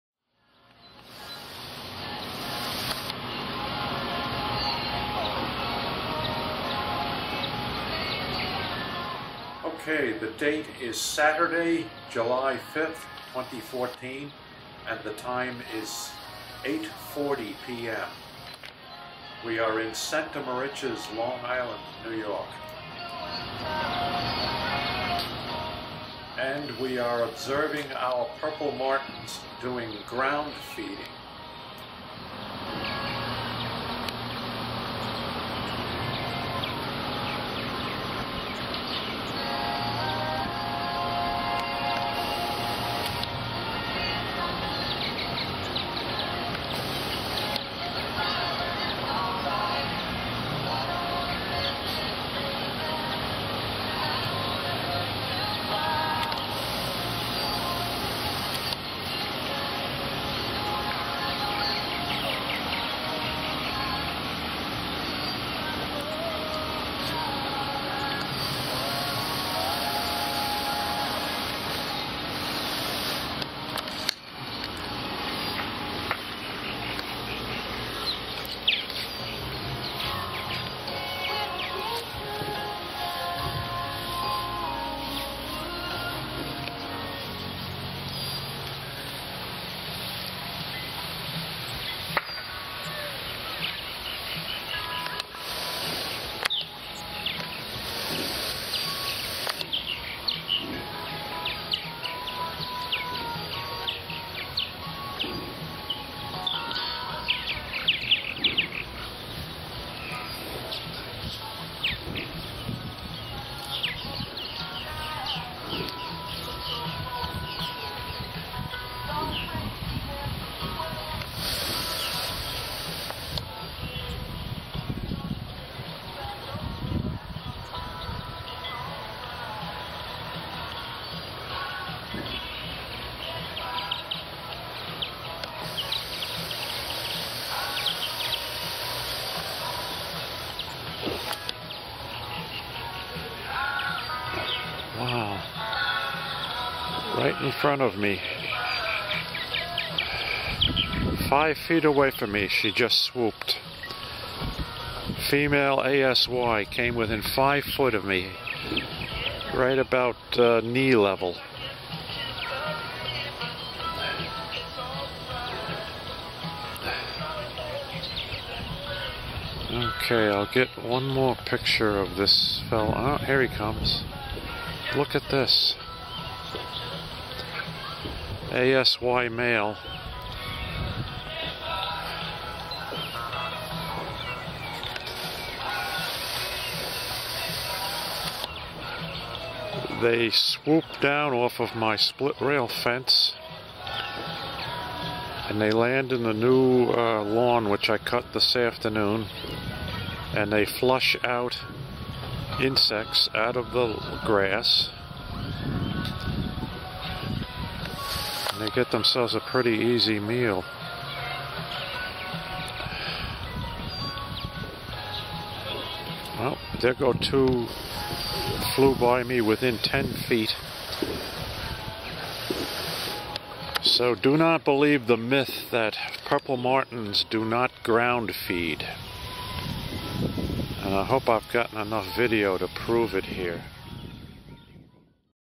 Purple Martins Ground Feeding in Center Moriches on July 5, 2014.
Saturday July 5, 2014 at 8:40 PM.

We observe our Purple Martins ground feeding this evening.  I have been observing this July evening event over the past 25+ years at our home, but this is the first time I have caught it on video.